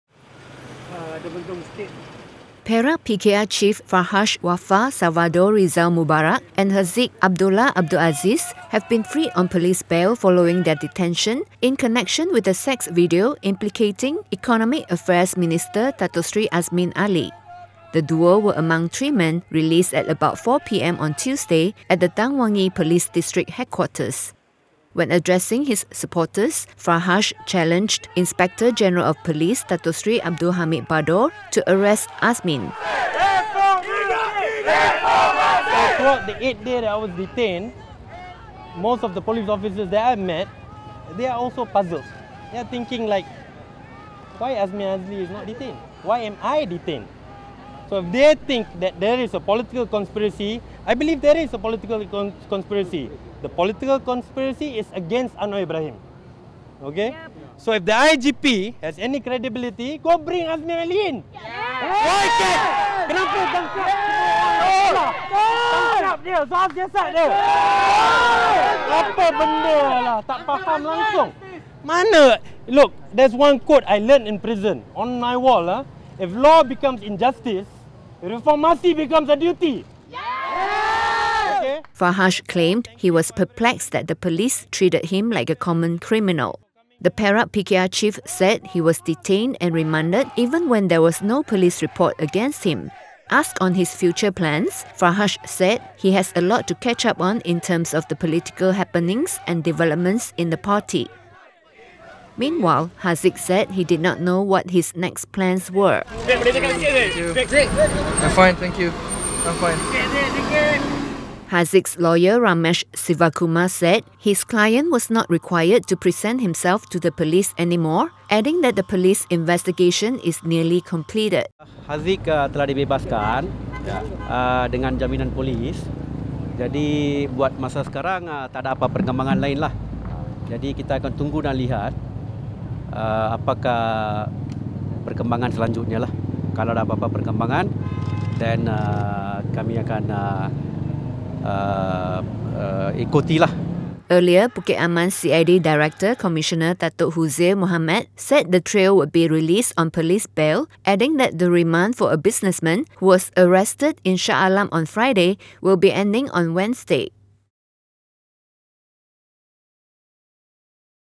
Sex video: Farhash, Haziq freed on police bail
Perak PKR chief Farhash Wafa Salvador Rizal Mubarak and Haziq Abdullah Abdul Aziz have been freed on police bail following their detention in connection with the sex video allegedly implicating Economic Affairs Minister Datuk Seri Azmin Ali.

They were among three men released at about 4pm on Tuesday at the Dang Wangi police district headquarters.